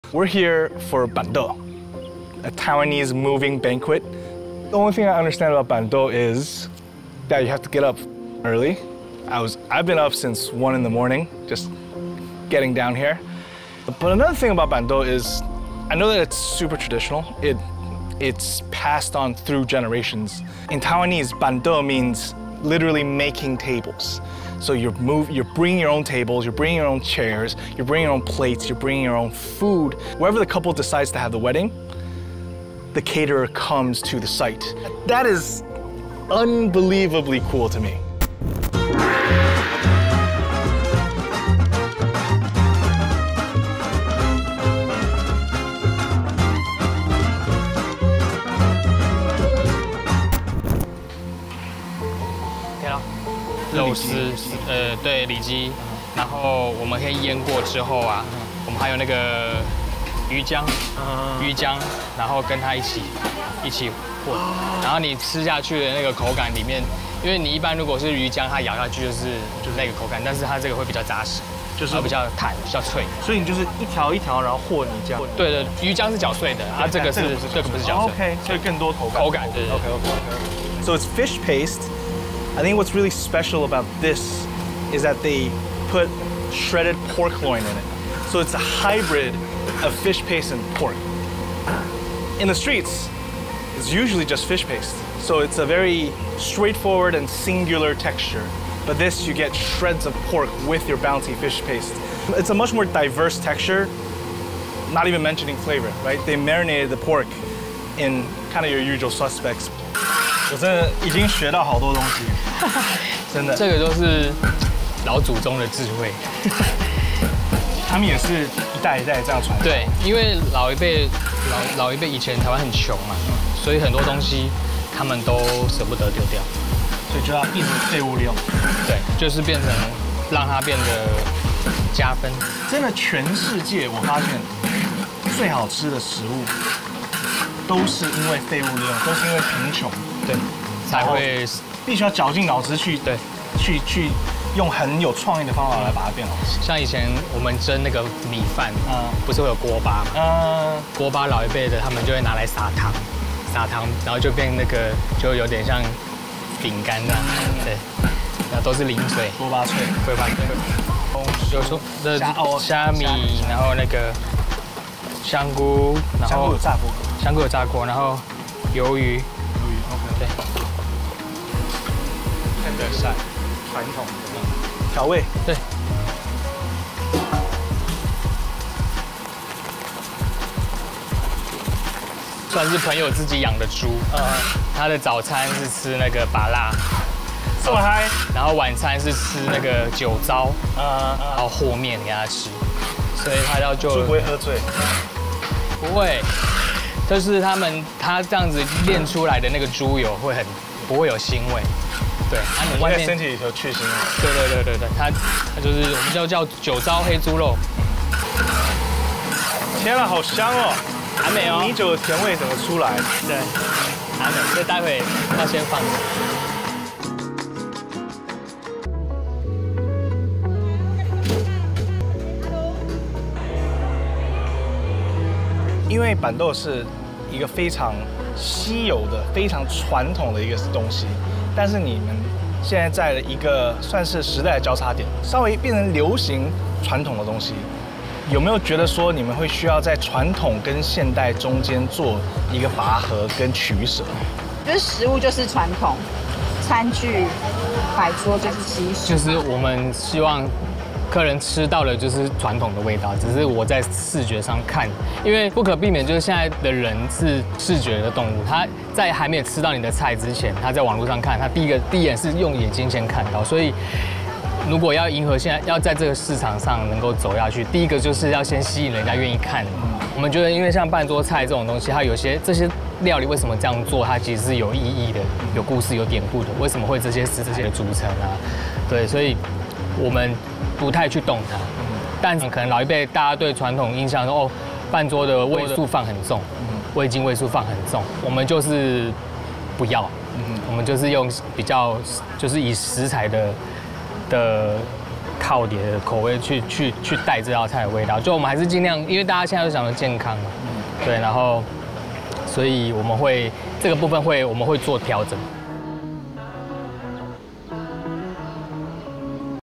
Bando: Taiwan’s Movable Feasts｜𝙏𝙖𝙞𝙬𝙖𝙣 𝘽𝙞𝙩𝙚𝙨
Bando (辦桌, Pan-toh) is a Taiwanese community and culinary tradition — a moving banquet.

Once very popular for large events like weddings, funerals, or celebrations, as a way to bring people together.

Let’s check some of its history and traditional dishes.

And see how young people today are innovating it.

🇹🇼 Curious about Taiwan’s culture? Planning a visit?

🌏 Want to stay up to date on the latest news from Taiwan and beyond?